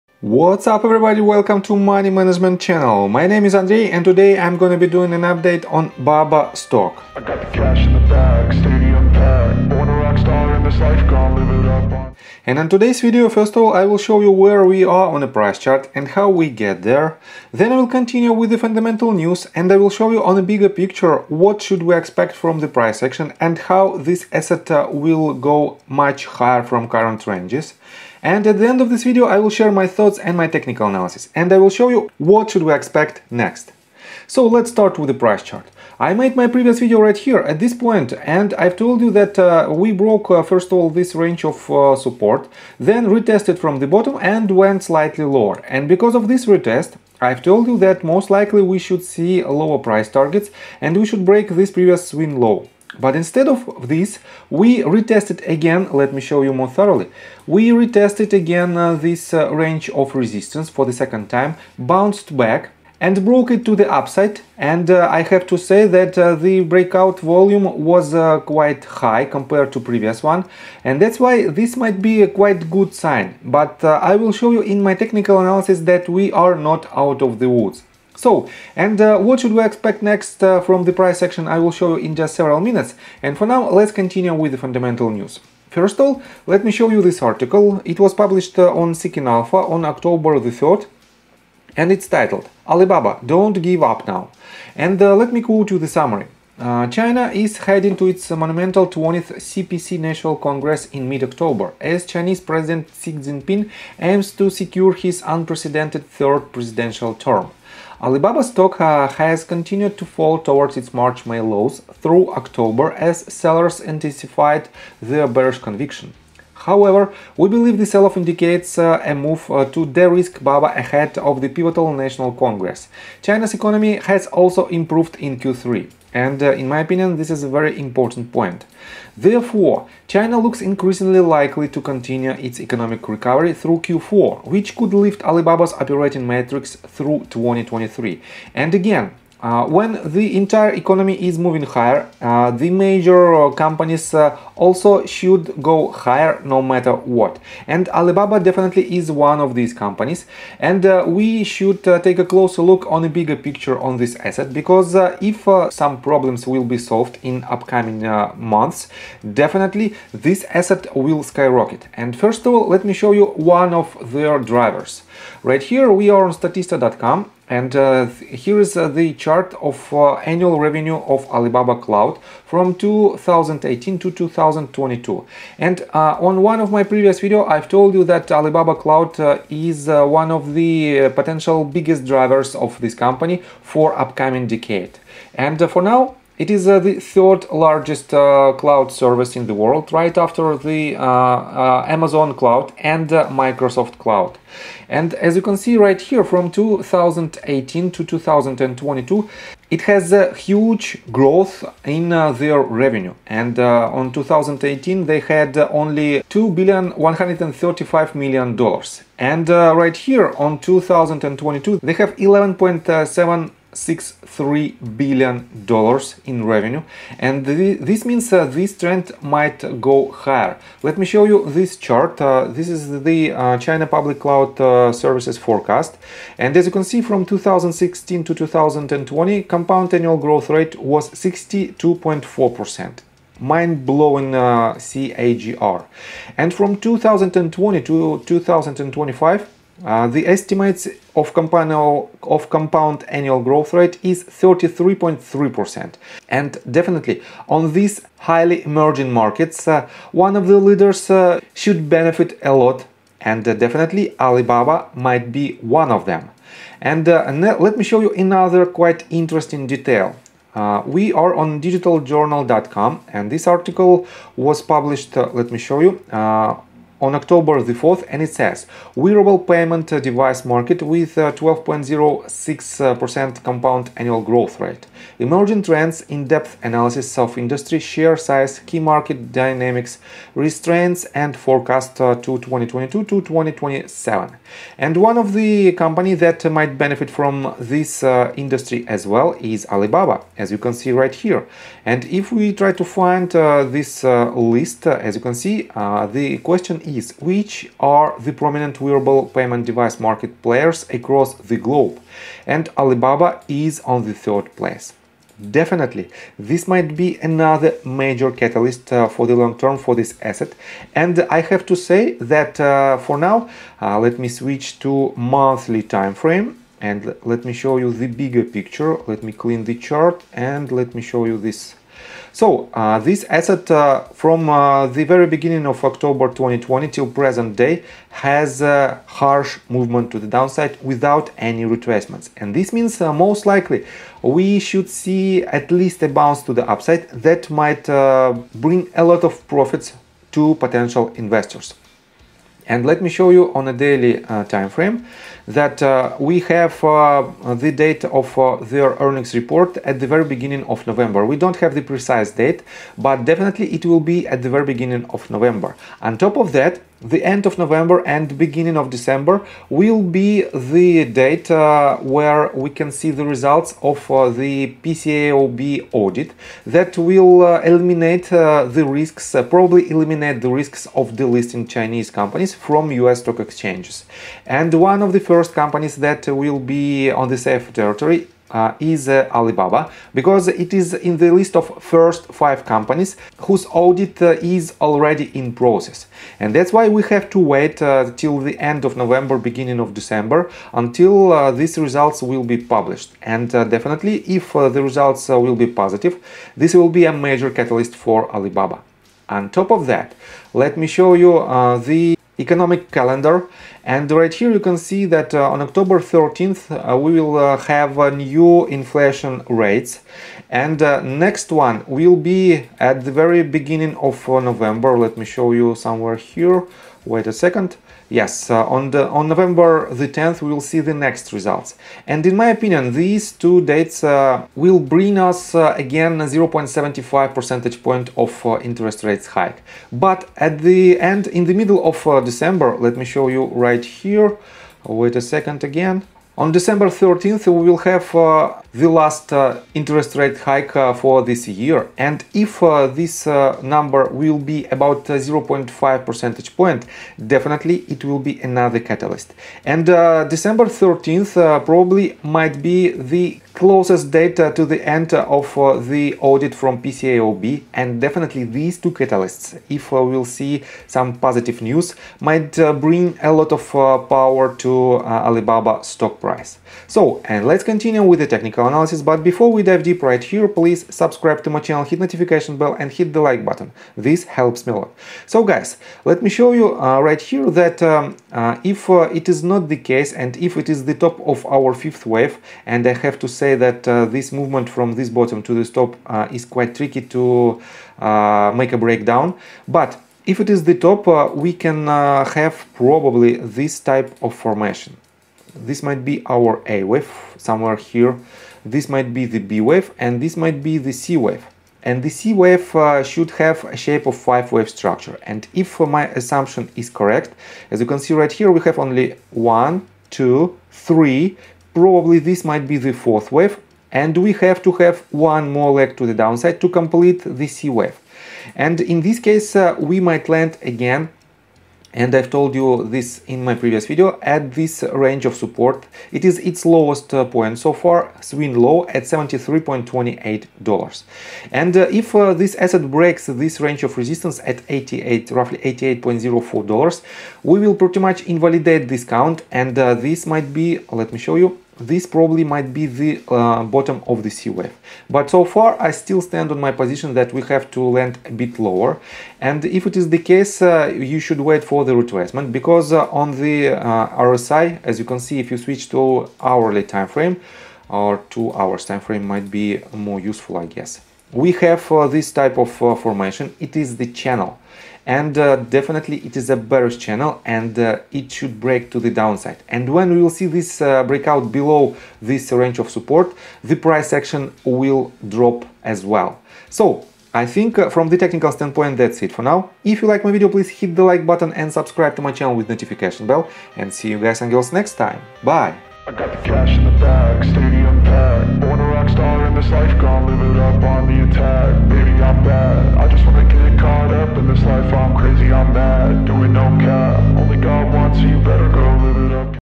BABA Stock PRICE PREDICTION! FOUR MAJOR CATALYSTS THAT WILL DRIVE BABA PRICE HIGHER!
👉Get 2 Free Stocks on Moomoo when you deposit $1+ (valued up to $7,000):

Alibaba Group Holding Limited, through its subsidiaries, provides online and mobile commerce businesses in the People's Republic of China and internationally. It operates through four segments: Core Commerce, Cloud Computing, Digital Media and Entertainment, and Innovation Initiatives and Others. In this BABA Stock Analysis video, we discuss swing trading and investing in the company. We analyze the 55 EMA (Exponential Moving average), MACD (Moving Average Convergence Divergence), RSI (Relative Strength Index), bullish and bearish trends, technical analysis chart patterns, and potential entry and exit points for traders and investors.

Timecodes:
0:00 Intro
0:11 Fundamental Analysis
7:47 Technical Analysis